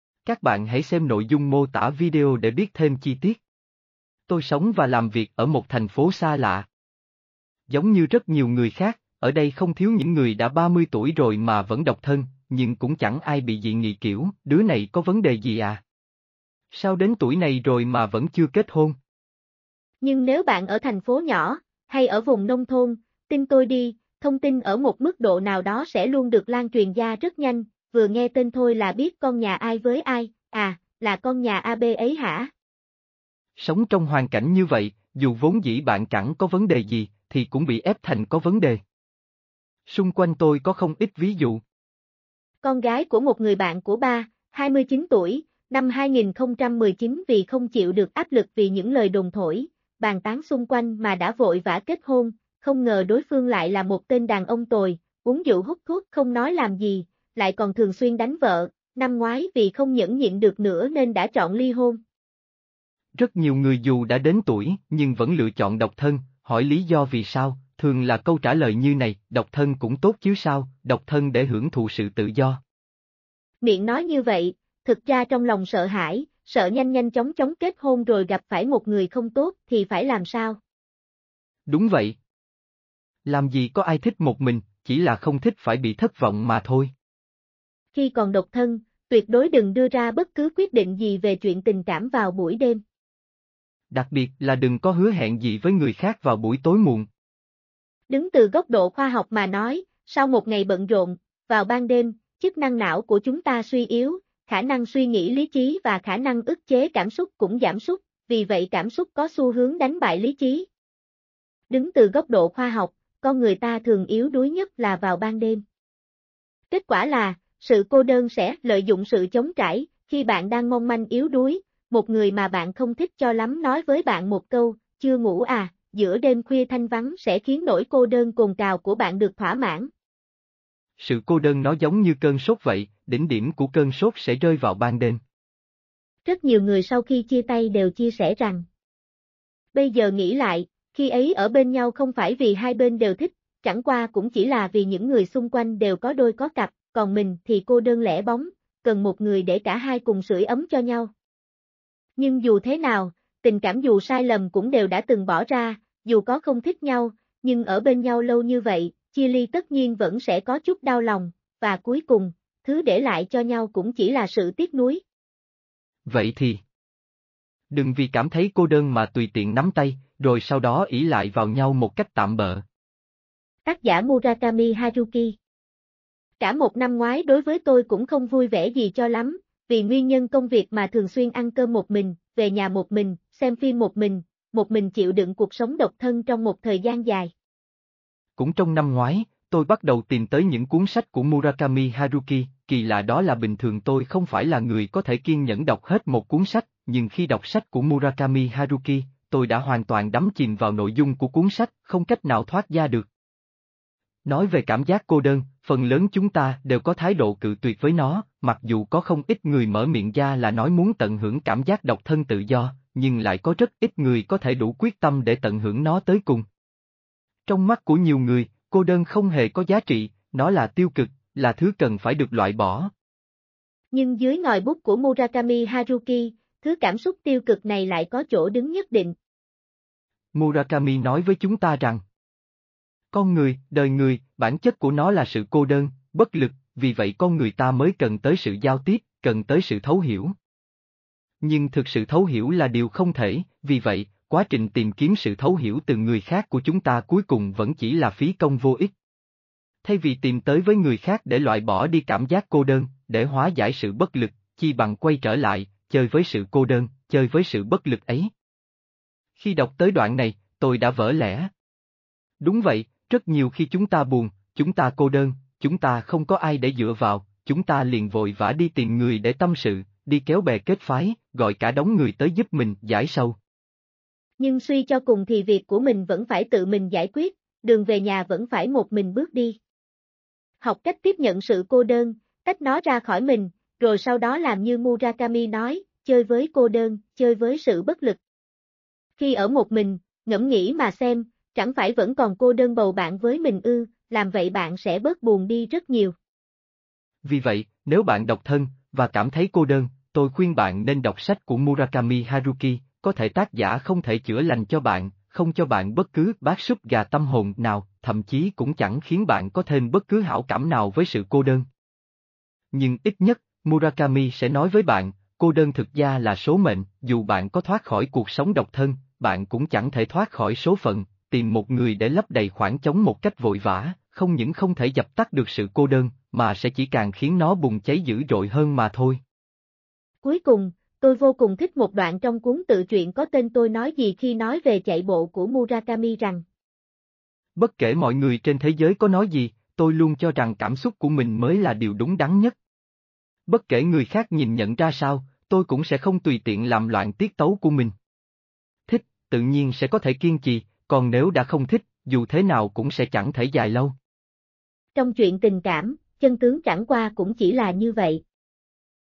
30 tuổi vẫn độc thân, hãy đọc Murakami Haruki: Đừng vì cô đơn quá lâu mà bước vào một mối quan h...
Tôi sống và làm việc ở một thành phố xa lạ.
Giống như rất nhiều người khác, ở đây không thiếu những người đã 30 tuổi rồi mà vẫn độc thân, nhưng cũng chẳng ai bị dị nghị kiểu: "Đứa này có vấn đề gì à? Sao đến tuổi này rồi mà vẫn chưa kết hôn?".
Nhưng nếu bạn ở thành phố nhỏ, hay ở vùng nông thôn, tin tôi đi, thông tin ở một mức độ nào đó sẽ luôn được lan truyền ra rất nhanh, vừa nghe tên thôi là biết con nhà ai với ai, "À, là con nhà AB ấy hả?".
Sống trong hoàn cảnh như vậy, dù vốn dĩ bạn chẳng có vấn đề gì, thì cũng bị ép thành có vấn đề.
Xung quanh tôi có không ít ví dụ.
Con gái của một người bạn của ba, 29 tuổi, năm 2019 vì không chịu được áp lực vì những lời đồn thổi, bàn tán xung quanh mà đã vội vã kết hôn, không ngờ đối phương lại là một tên đàn ông tồi, uống rượu hút thuốc không nói làm gì, lại còn thường xuyên đánh vợ, năm ngoái vì không nhẫn nhịn được nữa nên đã chọn ly hôn.
Rất nhiều người dù đã đến tuổi nhưng vẫn lựa chọn độc thân, hỏi lý do vì sao, thường là câu trả lời như này: "Độc thân cũng tốt chứ sao, độc thân để hưởng thụ sự tự do".
Miệng nói như vậy, thực ra trong lòng sợ hãi, sợ nhanh nhanh chóng chóng kết hôn rồi gặp phải một người không tốt thì phải làm sao.
Đúng vậy,
"Làm gì có ai thích một mình, chỉ là không thích phải bị thất vọng mà thôi".
Khi còn độc thân, tuyệt đối đừng đưa ra bất cứ quyết định gì về chuyện tình cảm vào buổi đêm.
Đặc biệt là đừng có hứa hẹn gì với người khác vào buổi tối muộn.
Đứng từ góc độ khoa học mà nói, sau một ngày bận rộn, vào ban đêm, chức năng não của chúng ta suy yếu, khả năng suy nghĩ lý trí và khả năng ức chế cảm xúc cũng giảm sút, vì vậy cảm xúc có xu hướng đánh bại lý trí.
Đứng từ góc độ khoa học, con người ta thường yếu đuối nhất là vào ban đêm.
Kết quả là, sự cô đơn sẽ "lợi dụng sự trống trải" khi bạn đang mong manh yếu đuối, một người mà bạn không thích cho lắm nói với bạn một câu "Chưa ngủ à?" giữa đêm khuya thanh vắng sẽ khiến nỗi cô đơn cồn cào của bạn được thỏa mãn.
Sự cô đơn nó giống như cơn sốt vậy, đỉnh điểm của cơn sốt sẽ rơi vào ban đêm.
Rất nhiều người sau khi chia tay đều chia sẻ rằng:
"Bây giờ nghĩ lại, khi ấy ở bên nhau không phải vì hai bên đều thích, chẳng qua cũng chỉ là vì những người xung quanh đều có đôi có cặp, còn mình thì cô đơn lẻ bóng, cần một người để cả hai cùng sưởi ấm cho nhau".
Nhưng dù thế nào, tình cảm dù sai lầm cũng đều đã từng bỏ ra, dù có không thích nhau, nhưng ở bên nhau lâu như vậy, chia ly tất nhiên vẫn sẽ có chút đau lòng, và cuối cùng, thứ để lại cho nhau cũng chỉ là sự tiếc nuối.
Vậy thì,
"Đừng vì cảm thấy cô đơn mà tùy tiện nắm tay, rồi sau đó ỷ lại vào nhau một cách tạm bợ".
Tác giả Murakami Haruki
Cả một năm ngoái đối với tôi cũng không vui vẻ gì cho lắm, vì nguyên nhân công việc mà thường xuyên ăn cơm một mình, về nhà một mình, xem phim một mình, một mình chịu đựng cuộc sống độc thân trong một thời gian dài.
Cũng trong năm ngoái, tôi bắt đầu tìm tới những cuốn sách của Murakami Haruki, kì lạ đó là bình thường tôi không phải là người có thể kiên nhẫn đọc hết một cuốn sách, nhưng khi đọc sách của Murakami Haruki, tôi đã hoàn toàn đắm chìm vào nội dung của cuốn sách, không cách nào thoát ra được.
Nói về cảm giác cô đơn, phần lớn chúng ta đều có thái độ cự tuyệt với nó, mặc dù có không ít người mở miệng ra là nói muốn tận hưởng cảm giác độc thân tự do, nhưng lại có rất ít người có thể đủ quyết tâm để tận hưởng nó tới cùng. Trong mắt của nhiều người, cô đơn không hề có giá trị, nó là tiêu cực, là thứ cần phải được loại bỏ.
Nhưng dưới ngòi bút của Murakami Haruki, thứ cảm xúc tiêu cực này lại có chỗ đứng nhất định.
Murakami nói với chúng ta rằng:
"Con người, đời người, bản chất của nó là sự cô đơn, bất lực, vì vậy con người ta mới cần tới sự giao tiếp, cần tới